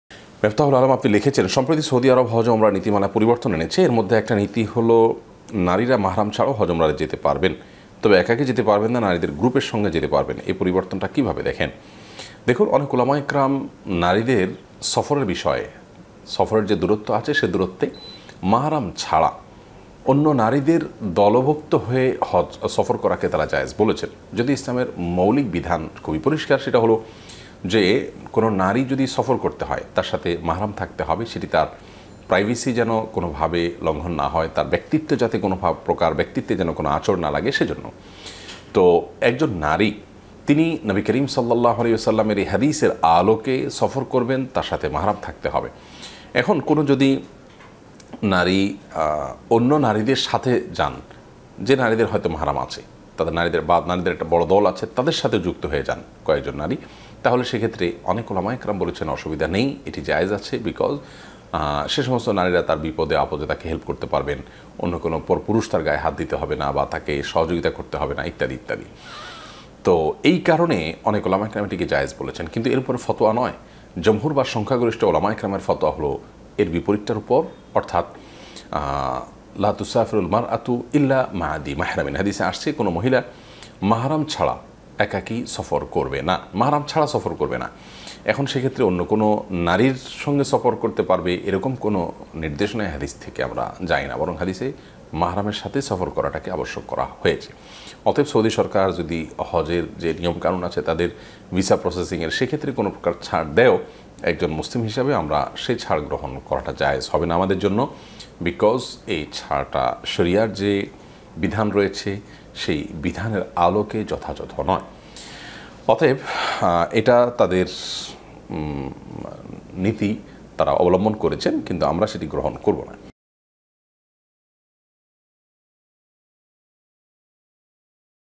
মাহরাম ব্যতিত নারীর একাকি হজে যাওয়ার শর'য়ী বিধান কী? শরয়ী সমাধান II Sheikh ahmadullah live
সৌদী সরকার এই বছর থেকে নারীদের একাকী হজে যাওয়ার সুযোগ রেখেছে,নারীরা মাহরাম ব্যতিত একাকী হজে যাওয়ার বিধান কী তারই শরয়ী সমাধান দিয়েছেন শায়খ আহমাদুল্লাহ II Sheikh ahmadullah live